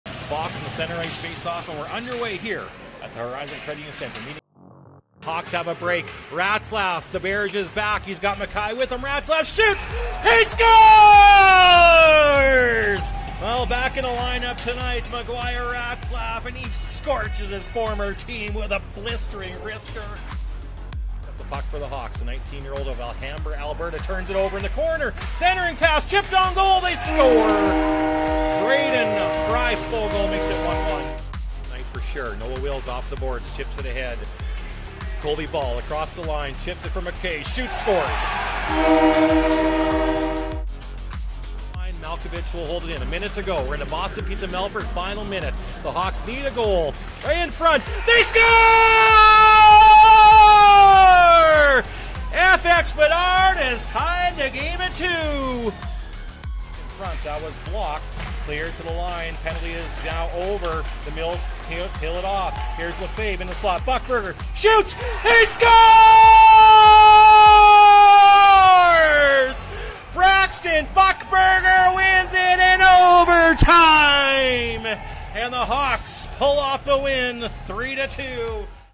Highlights: Nipawin Hawks vs Melville Millionaires Nov 27
The Nipawin Hawks earned a big two points on the road Sunday night with a 3-2 overtime win against the Melville Millionaires.